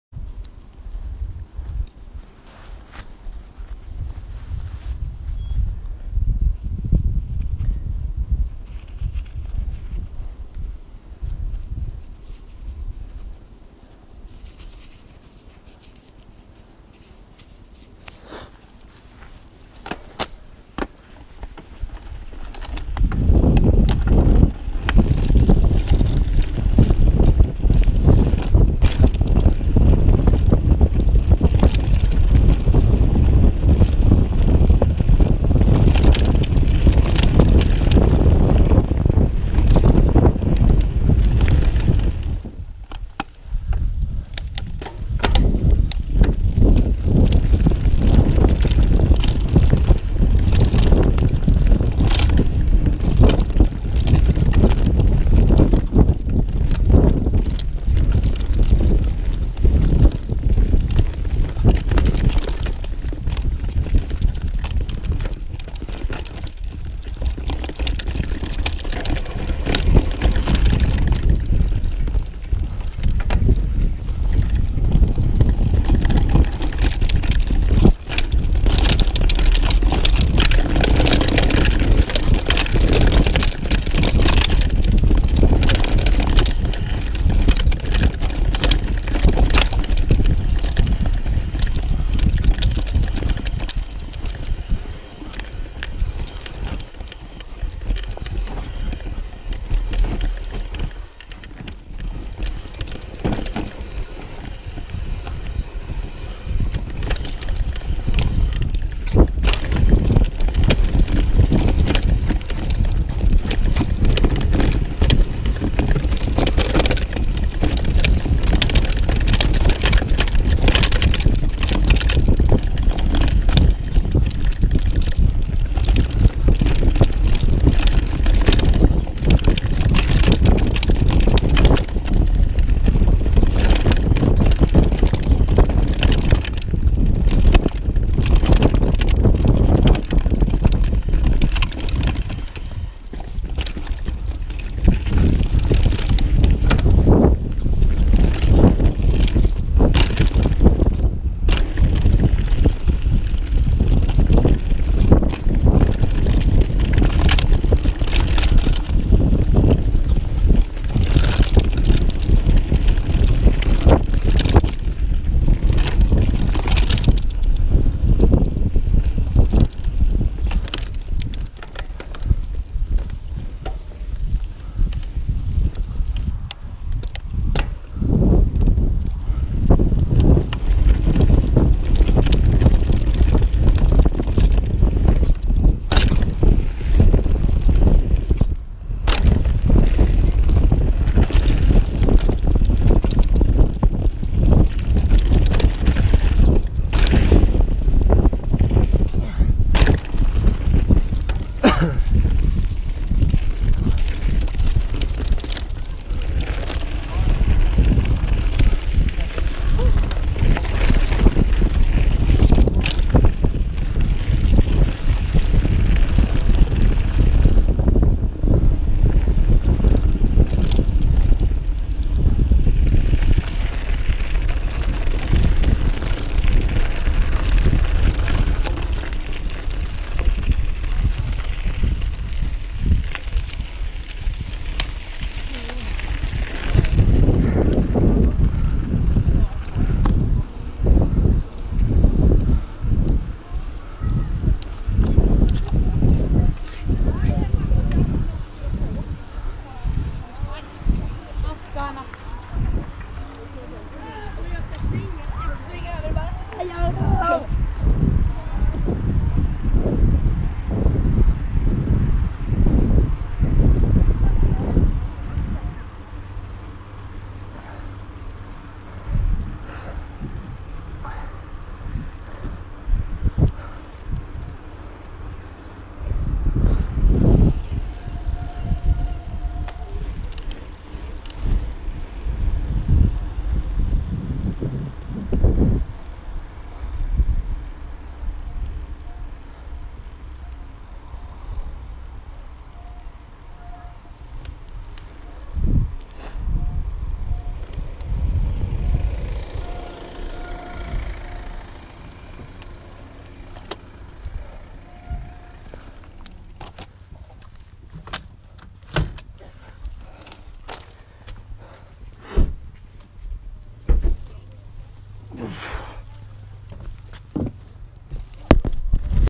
Isaberg 2019
Just a quick clip going down Isaberg